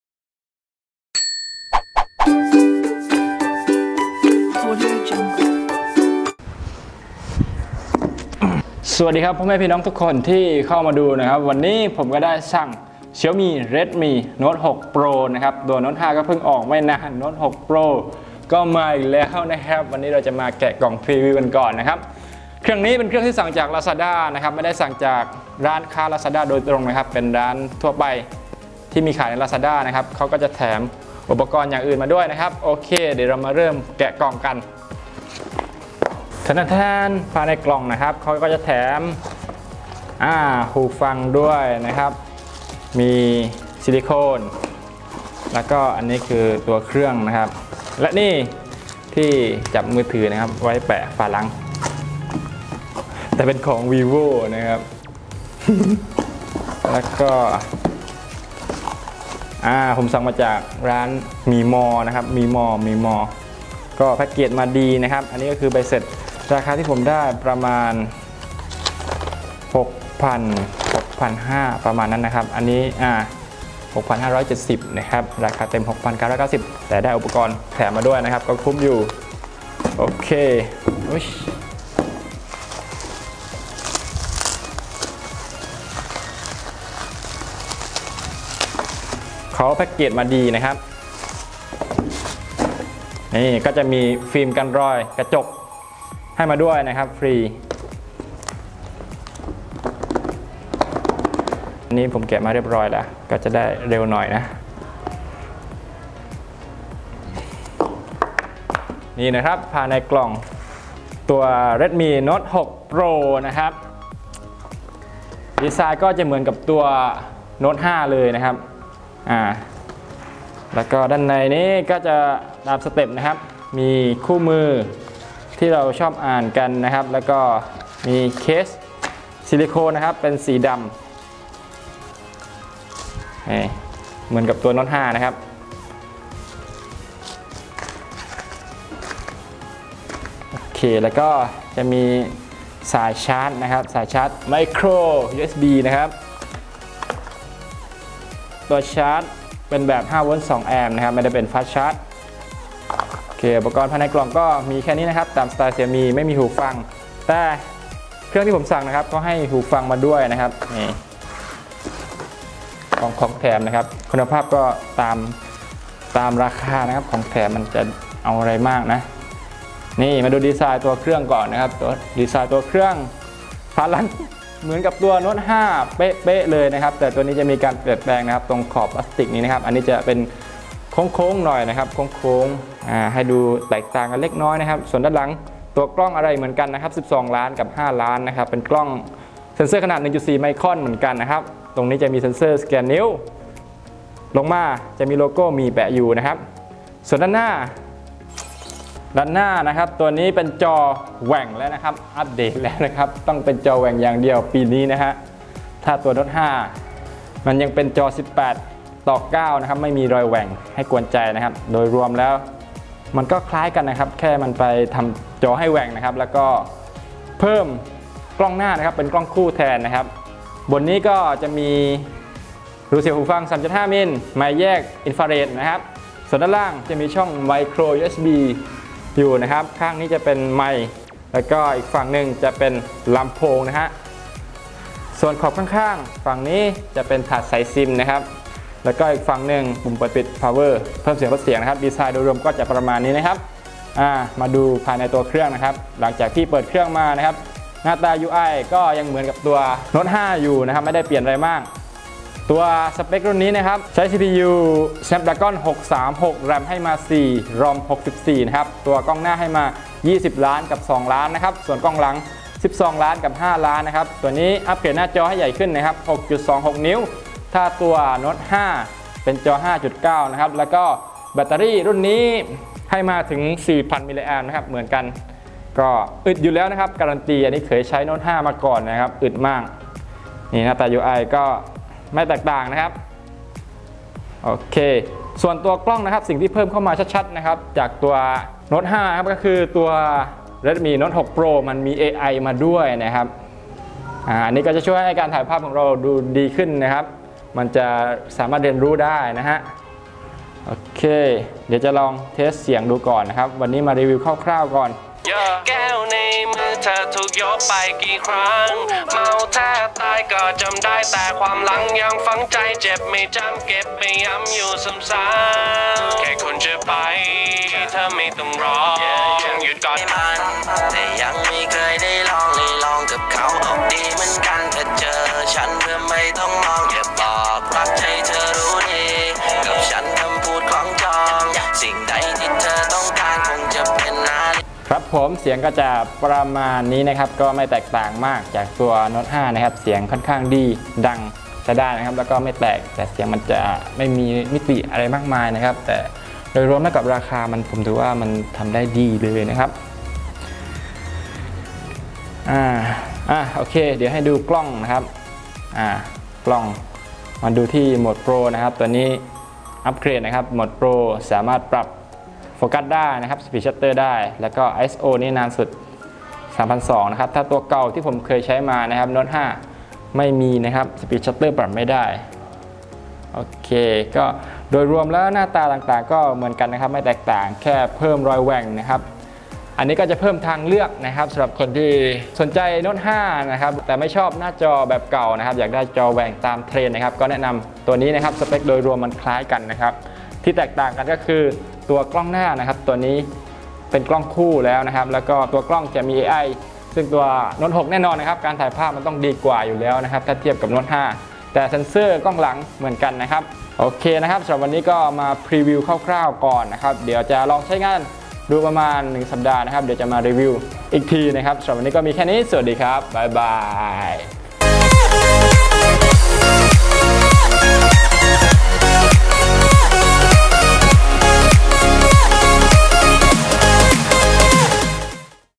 แกะกล่อง Xiaomi Redmi Note 6 Pro ราคาเดิม 6,990 บาท อัพเดตอะไรบ้าง!
สเปค REDMI NOTE 6 PRO
หน้าจอขนาด 6.26 นิ้ว Full HD+ อัตราส่วน 19:9
CPU : Snapdragon 636
RAM : 4GB
ความจุ : 64GB รองรับ MicroSD Card
กล้องหลังคู่ : 12MP ขนาด 1.4 ไมครอน + 5MP, ระบบ AI
กล้องหน้า : 20MP + 2MP และไฟเซลฟี่แฟลช
สแกนนิ้วมือด้านหลัง
รองรับ 2 SIM (ถาดซิม Hybrid slot)
การเชื่อมต่อ : MicroUSB
แบตเตอรี่ : 4000 mAh
ขนาด : 157.9 x 76.38 x 8.2 มม.
สีที่วางจำหน่าย : ดำ ทอง ทองกุหลาบ ฟ้า แดง